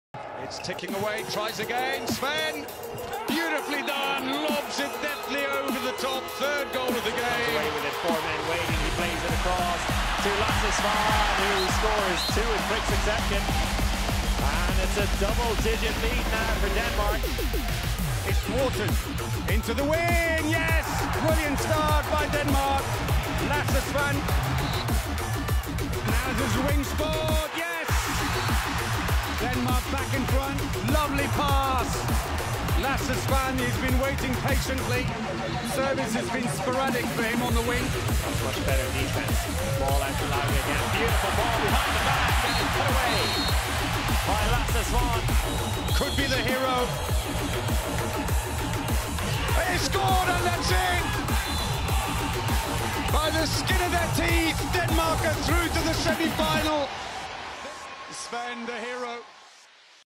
Lasse Svan will play his last match for Denmark this week
One of the most capped players in 🇩🇰 Denmark's history, Lasse Svan, will play his last match for the national team this week ⭐ And what a career 🙌

With Denmark, Svan has taken a collection of medals and been ✅ World champion x 2 ✅ Olympic champion ✅ European champion 👏